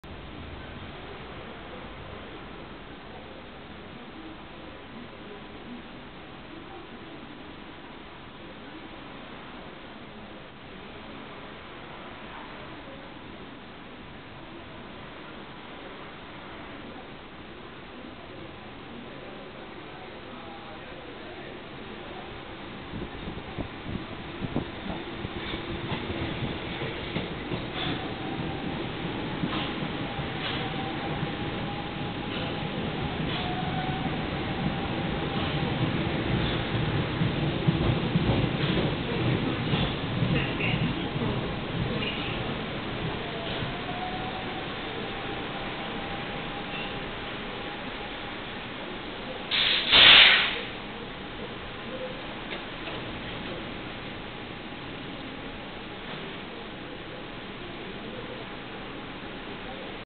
Mitsubishis en la estación Carlos Pellegrini (11): Servicio limitado por el Mini-Davos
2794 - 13 de septiembre de 2016 - METROVÍAS/Línea B. Como consecuencia del operativo de seguridad que tiene lugar en el Bajo porteño por el Foro de Inversiones y Negocios "Mini Davos" en el ex Correo Central, las estaciones Florida y Leandro N. Alem de la Línea B permanecen cerradas, restringiendo el servicio entre Carlos Pellegrini y Villa Urquiza (Juan Manuel de Rosas).

En el video puede verse a la formación Mitsubishi Eidan Q (741, 907, 785, 786, 908 y 724) arribando a la estación Carlos Pellegrini y completando su recorrido.